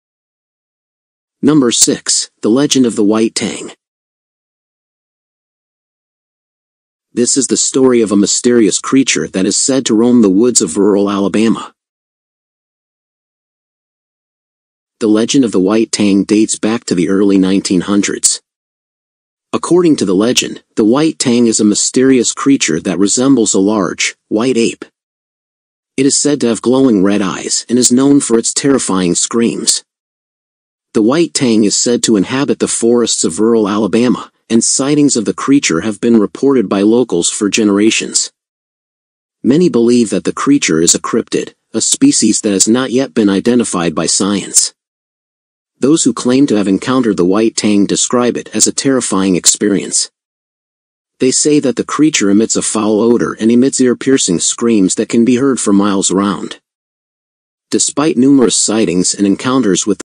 Unmasking the Alabama White Thang: The Mysterious Creature That Haunts the Shadows
Enter the realm of the unknown as we delve into the chilling lore surrounding the enigmatic creature known as the Alabama White Thang. Join us on a hair-raising exploration of this mysterious cryptid that lurks in the shadows, captivating the imaginations of locals and paranormal enthusiasts alike. From eerie sightings to unsettling encounters, witness the firsthand accounts and uncover the truth behind this elusive being. Brace yourself for a journey into the depths of Alabama's supernatural legends as we unravel the secrets and attempt to shed light on the existence of the White Thang. Are you ready to confront the mystery?